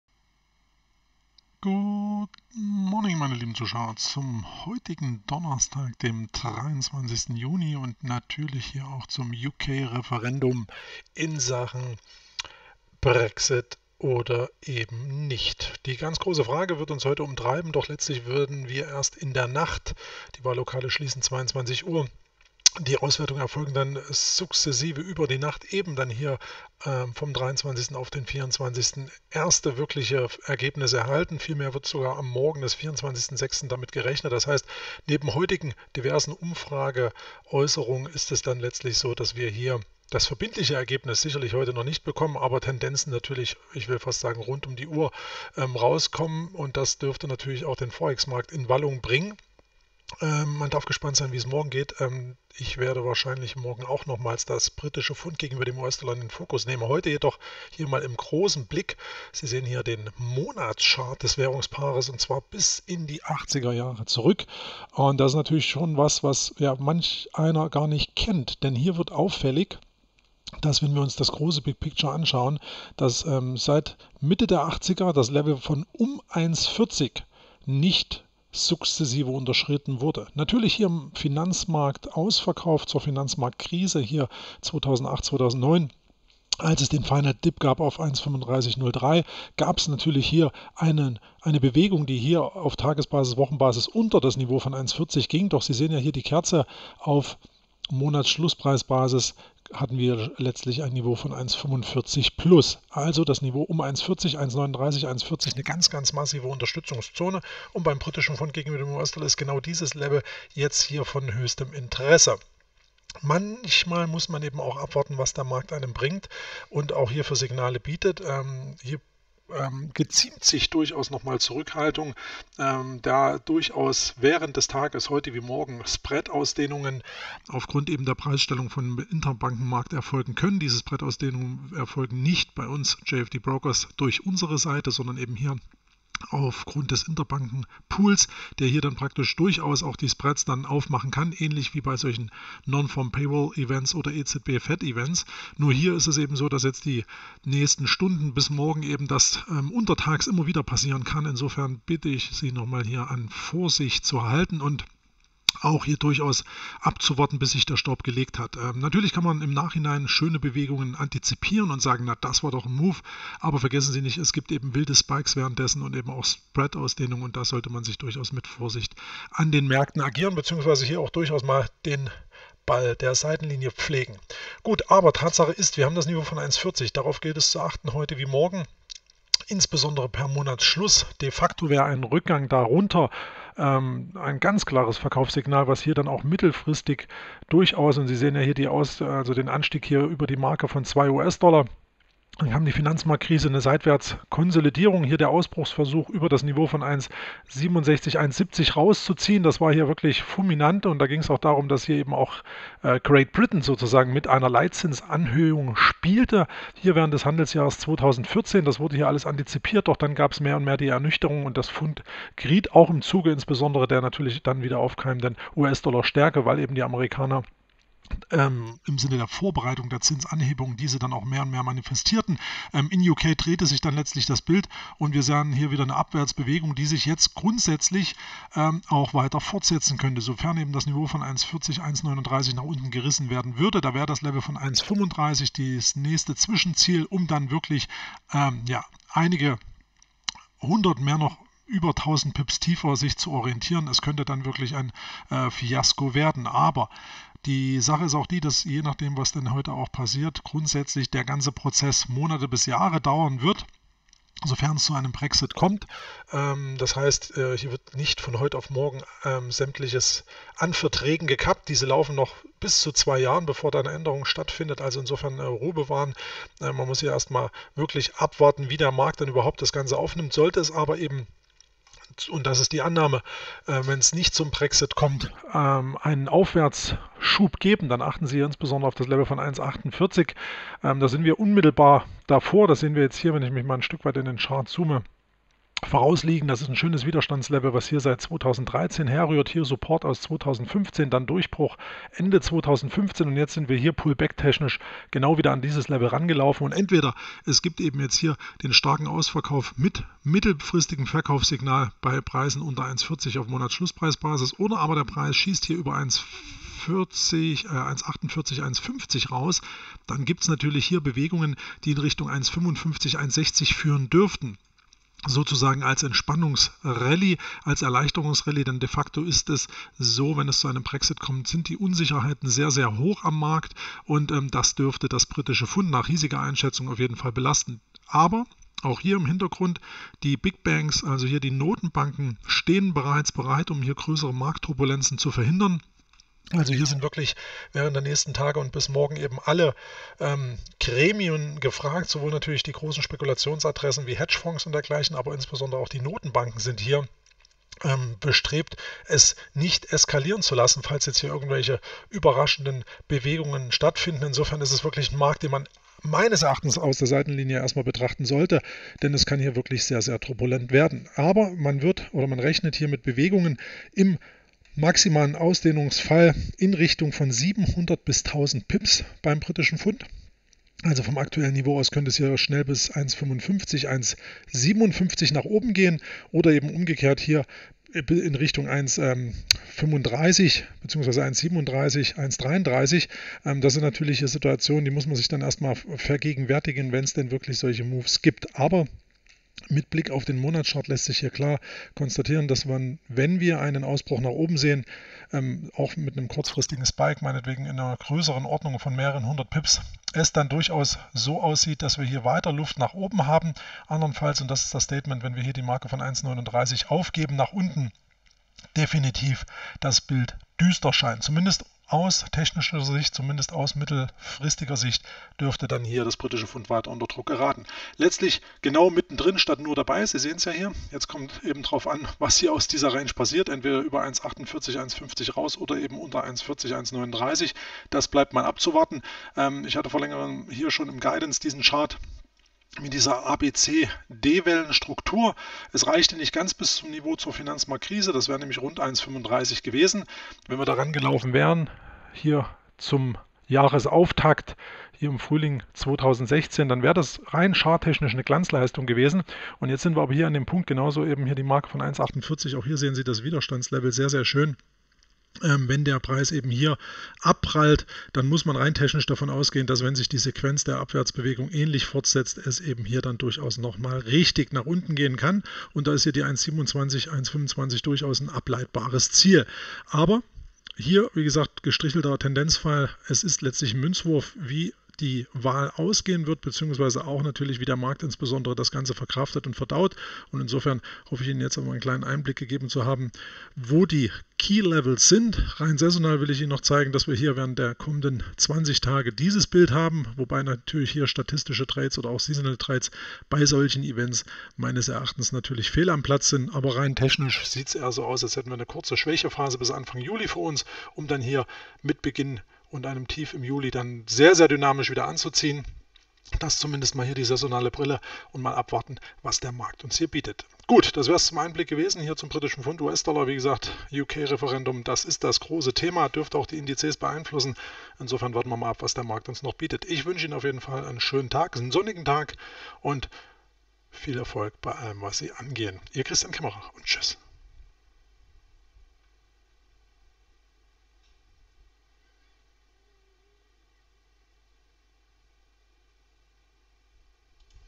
JFD Devisenradar: GBP/USD im Blick zum Start ins Referendum
Heute könnten, nach ersten Hochrechnungen, bereits einige äußerst wilde Ausschläge erfolgen. Letztlich wird sich jedoch erst in der Nacht auf den 24.06. ein entgültiges Ergebnis ableiten und somit bleibt die Spannung groß.